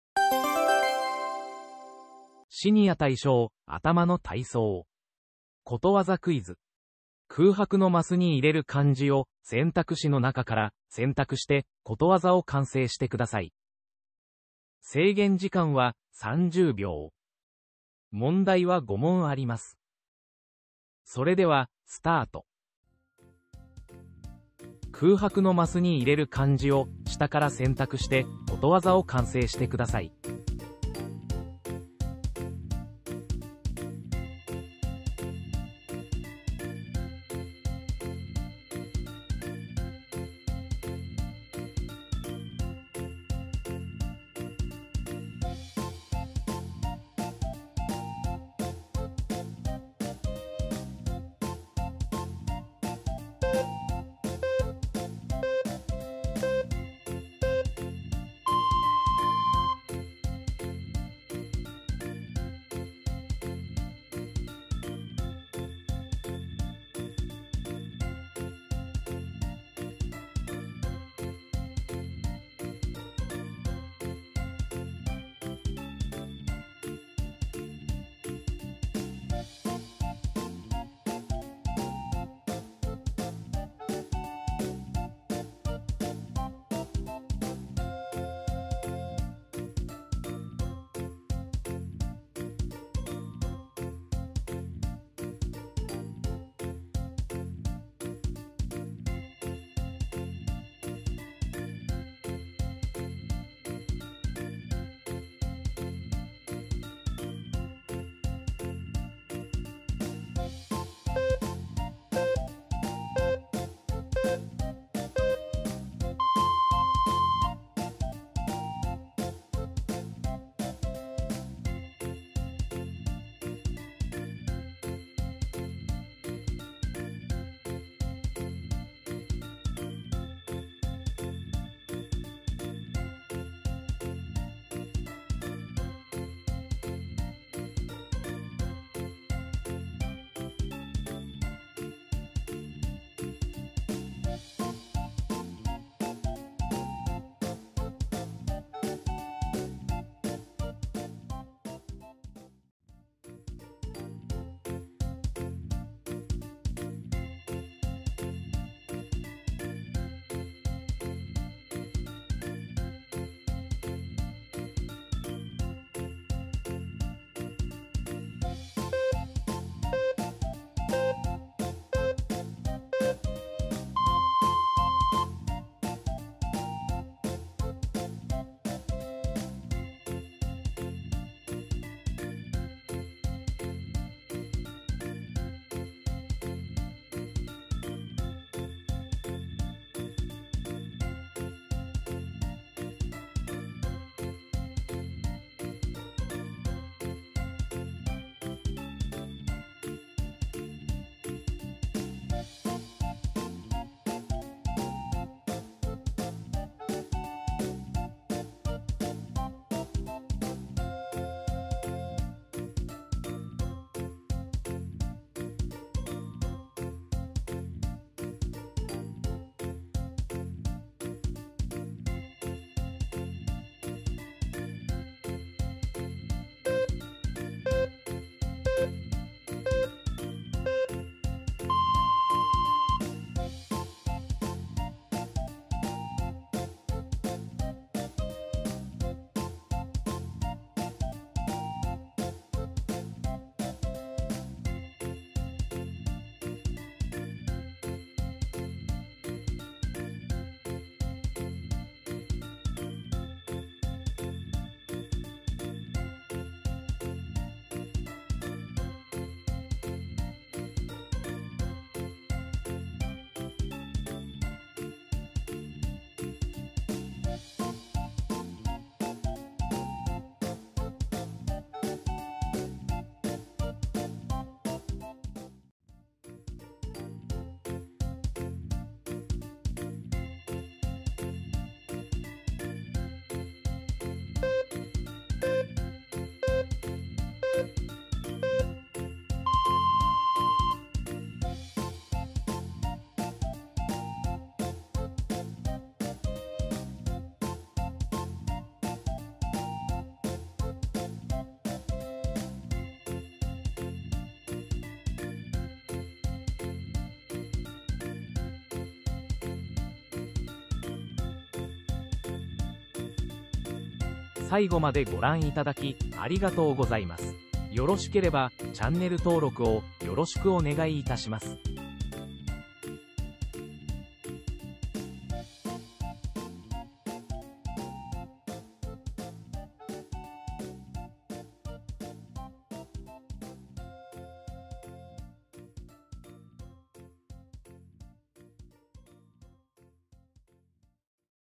シニア向け！脳トレ ことわざクイズ 003
この動画は、シニアの方を対象とした脳トレクイズ動画です。

音楽素材
フリーBGM DOVA-SYNDROME
画像素材
イラストAC